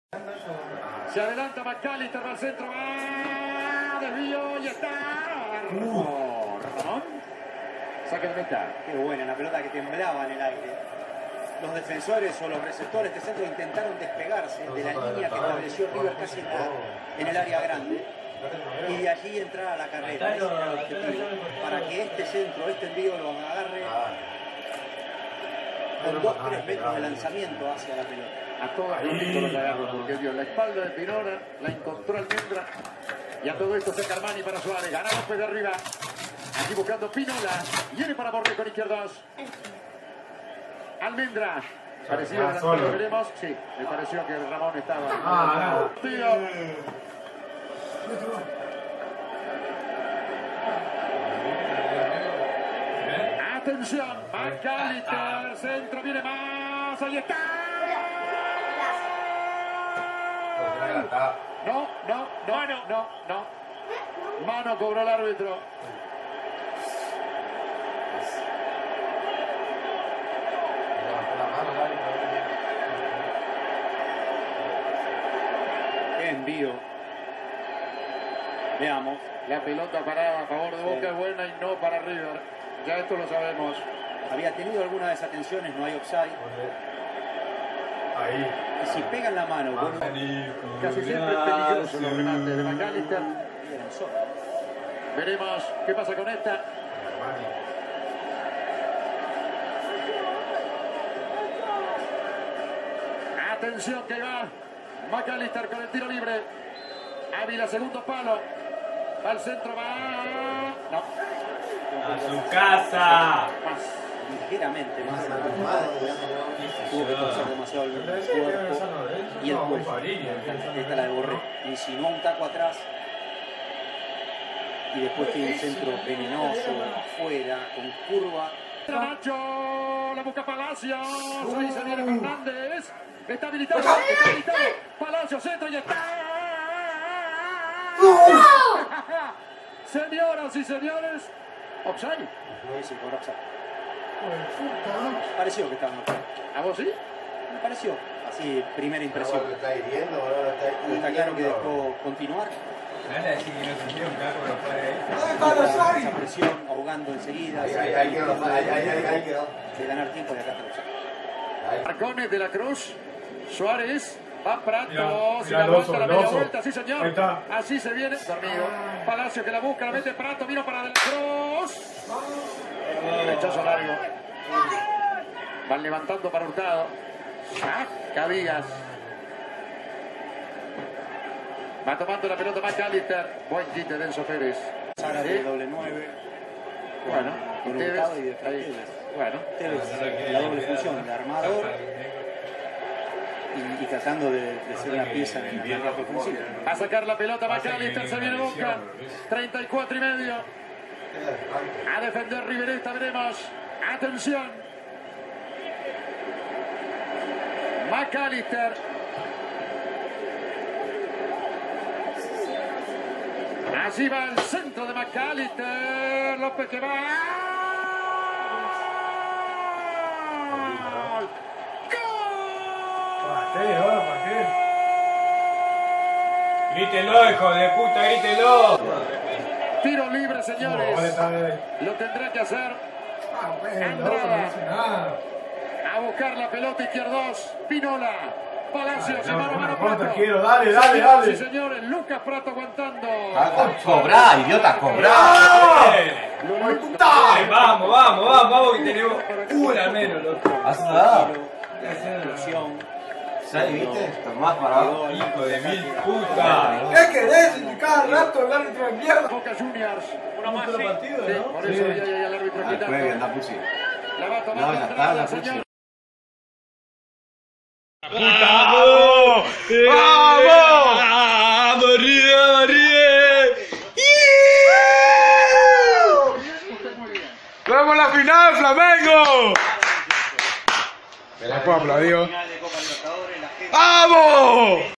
BOCA JUNIORS VS RIVER PLATE/REACCIÓN DE HINCHA DE RIVER/SEMIFINAL VUELTA / COPA LIBERTADORES 2019
A por Flamengo